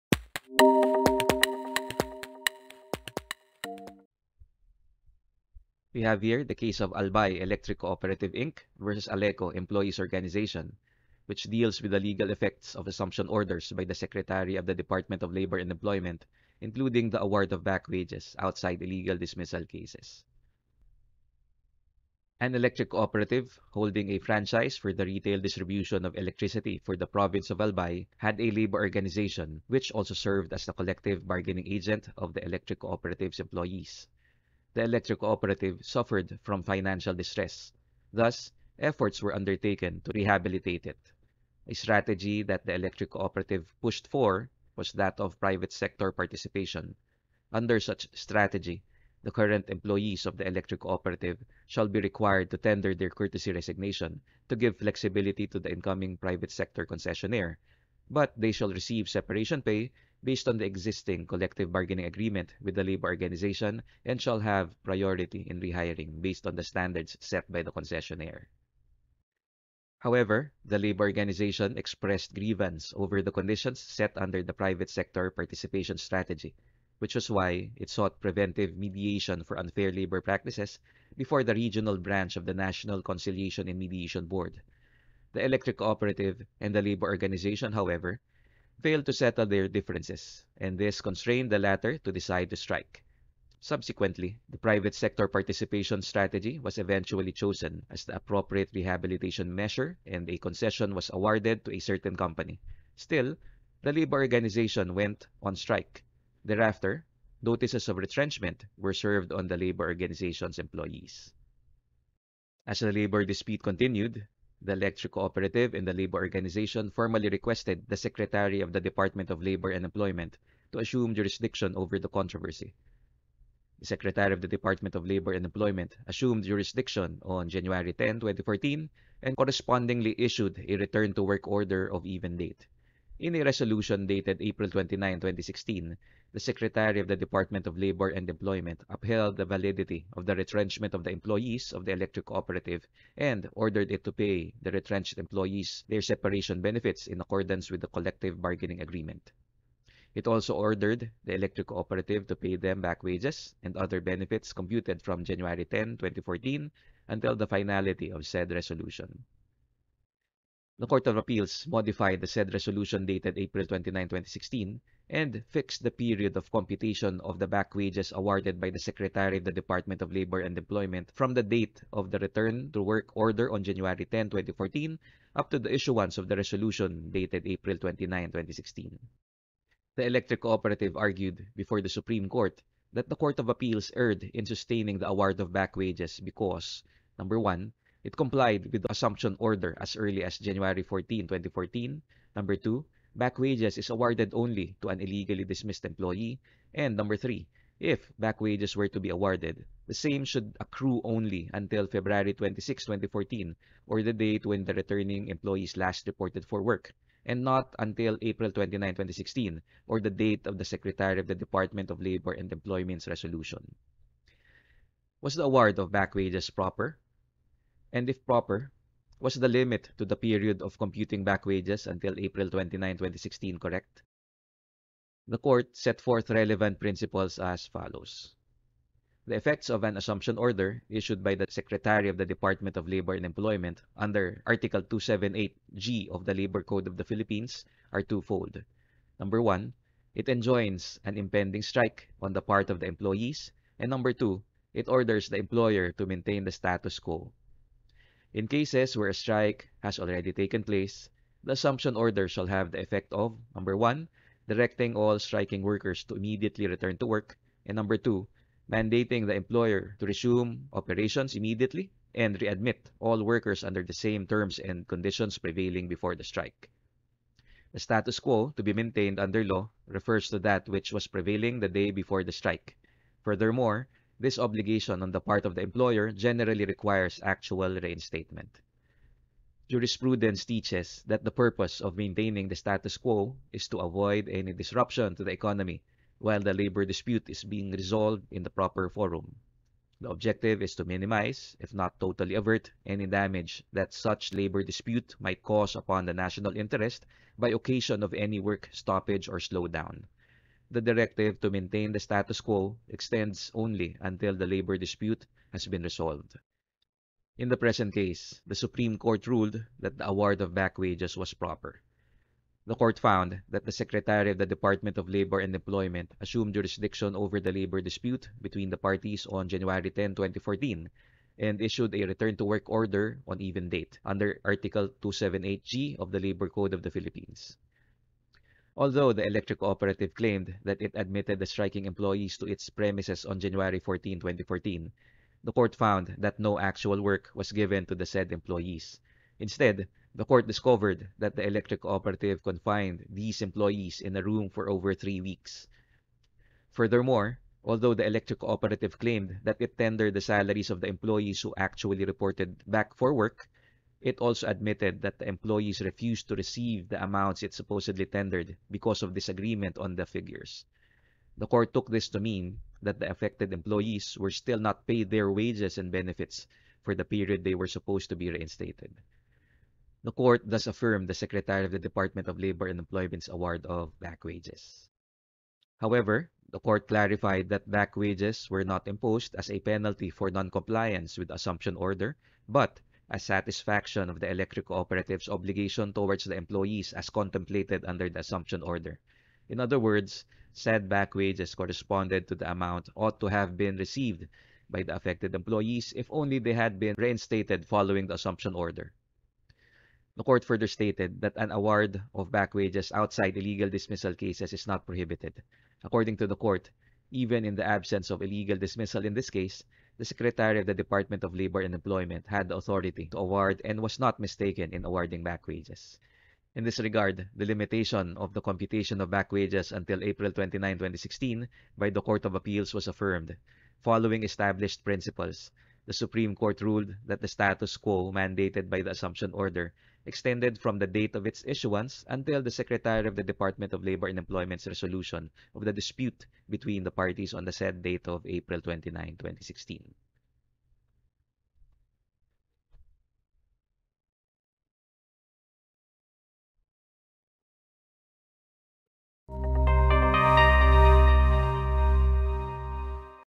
But “Backwages” Is Awarded Only to an Illegally Dismissed Employee
In a case, the Supreme Court reiterated the principles relating to the powers of the Secretary of the Department of Labor and Employment in national interest disputes, including the related principle on the significance of maintaining the status quo.

Albay Electric Cooperative, Inc. v. ALECO Labor Employees Organization, G.R. No. 241437, September 14, 2020.